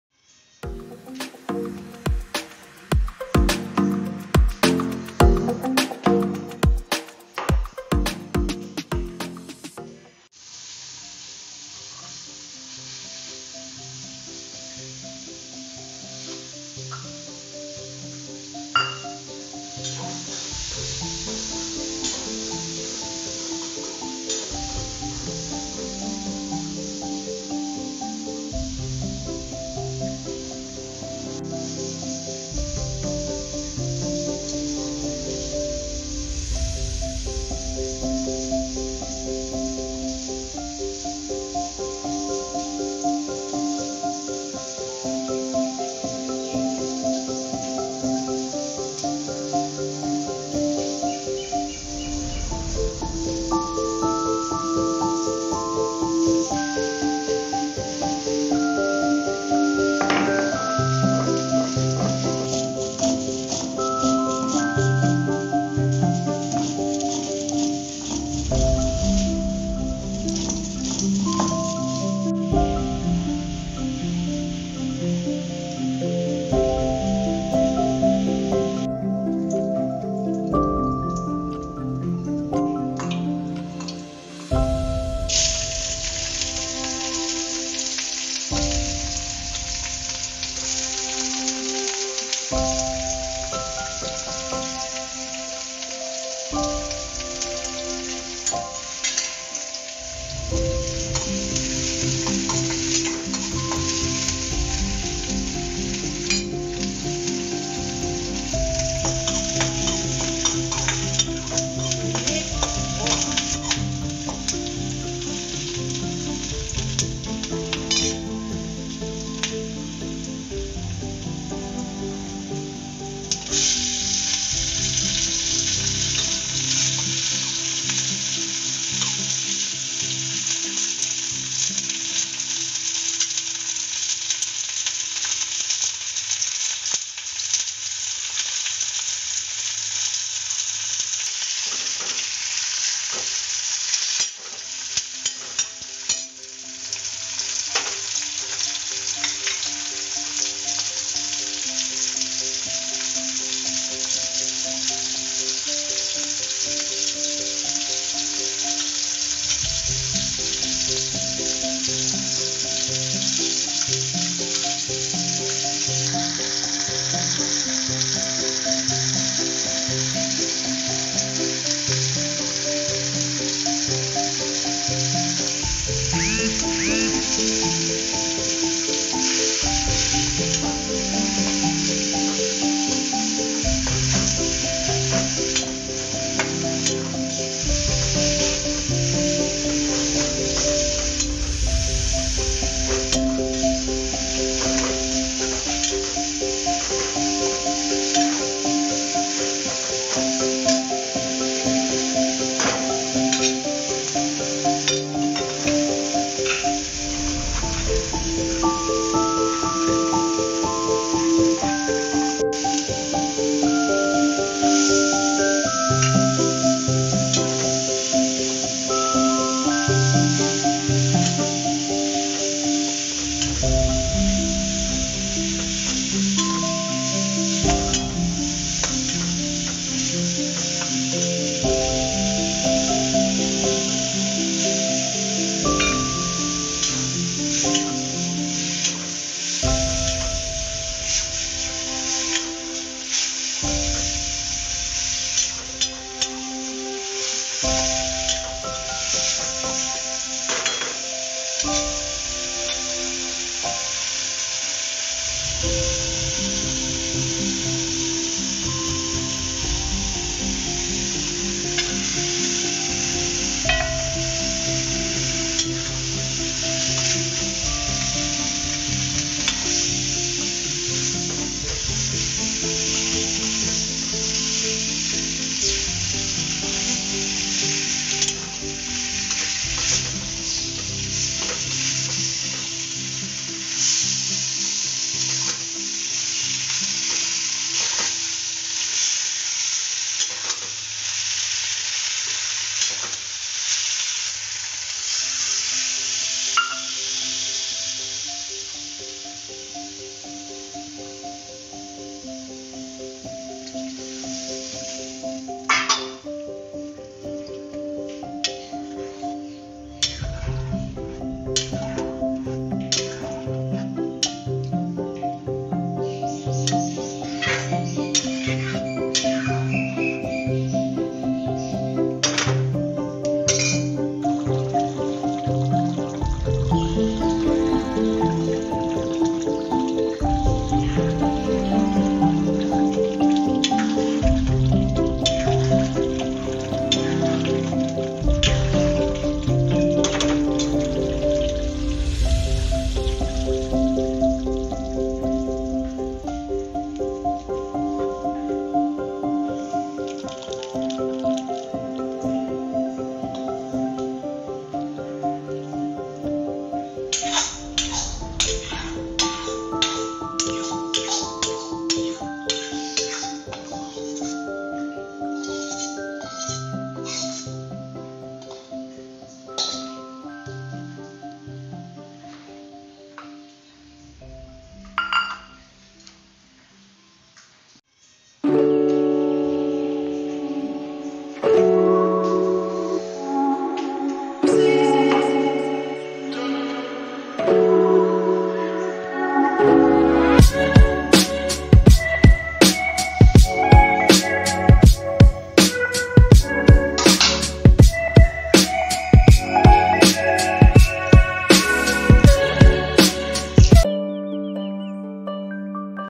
Chola Alur Dom | ছোলা আলুর দম | Chickpea potato curry | cholar tarkari | new delicious recipe 🤤💕
Hey guys!

Today's recipe is Chola alur dom ( chola alur torkari। / chickpea potato curry / chana alur torkari / chana alu ki sabji  ) how to make chola alur torkari?  Watch full recipe and tell us how's it in the comment section below.

Try it in your home trust me it's really delicious unique recipe. Also tag us in our Instagram page if you like our recipes. 🤗♥️